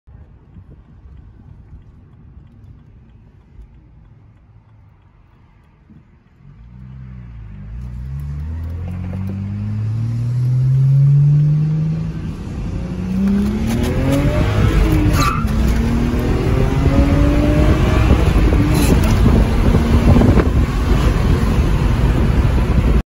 BMW 335i N54 manual (DCI, Downpipes) stock exhaust, no tune
BMW 335i N54 DCI and Downpipes with stock exhaust